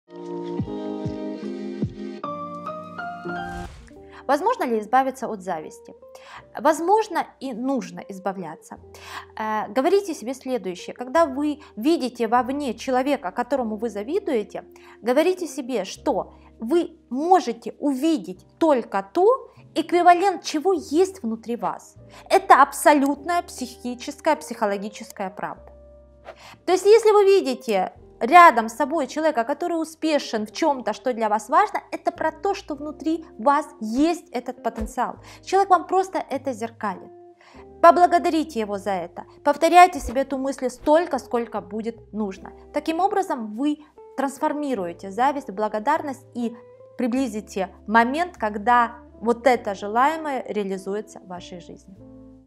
Как избавиться от зависти к людям? Советы психолога. Кристина Кудрявцева 18+
Как избавиться от зависти к людям? Возможно ли избавиться от зависти? Возможно, и нужно избавляться, говорите себе следующее, когда вы видите вовне человека, которому вы завидуете, говорите себе, что вы можете увидеть только то, эквивалент чего есть внутри вас. Как перестать завидовать. Это абсолютная психическая, психологическая правда, то есть, если вы видите рядом с собой человека, который успешен в чем-то, что для вас важно, это про то, что внутри вас есть этот потенциал, человек вам просто это зеркалит, поблагодарите его за это. Повторяйте себе эту мысль столько, сколько будет нужно, таким образом, вы трансформируете зависть, благодарность и приблизите момент, когда вот это желаемое реализуется в вашей жизни.

Интересные видео:
✅ Личный рост и развитие. "Живу хуже всех и нет сил это изменить"... Как поменять свою жизнь? Кристина Кудрявцева ➤

✔ Оставляйте своё мнение в комментариях!